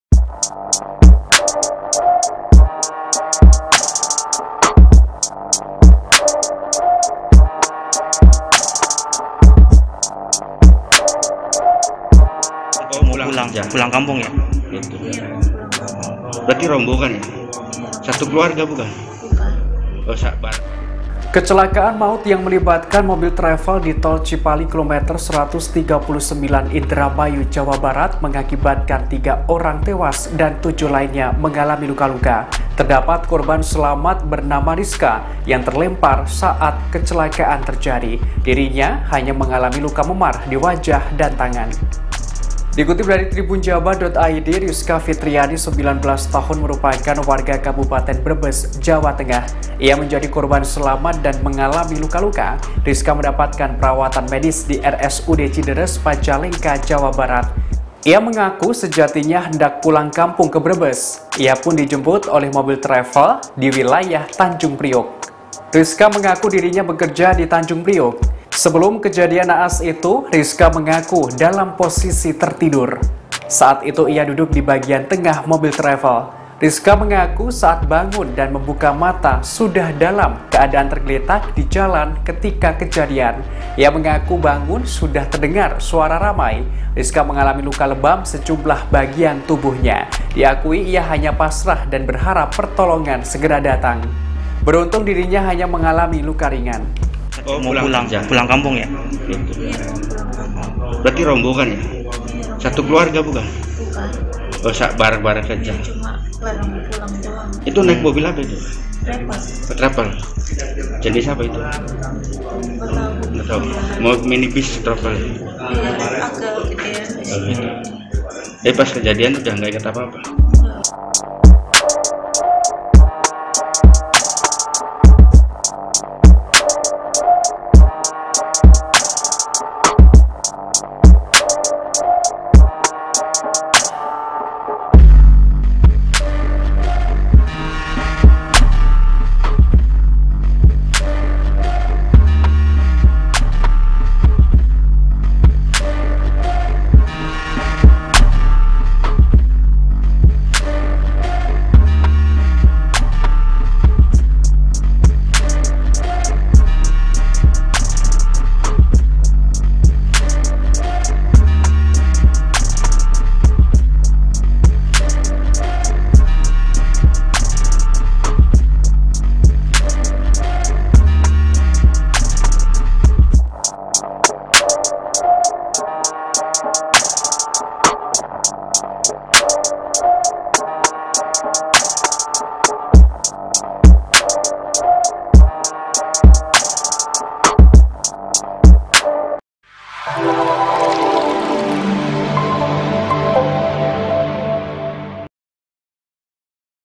CERITA Riska, Dalam Posisi Tertidur Dirinya Terlempar saat Kecelakaan Terjadi di Tol Cipali
Kecelakaan maut yang melibatkan mobil travel di Tol Cipali KM 139, Indramayu, Jawa Barat mengakibatkan 3 orang tewas dan 7 lainnya mengalami luka-luka.

Korban selamat bernama Riska menceritakan saat dirinya terlempar saat kecelakaan terjadi.

Riska mengaku hanya mengalami luka memar di wajah dan tangan.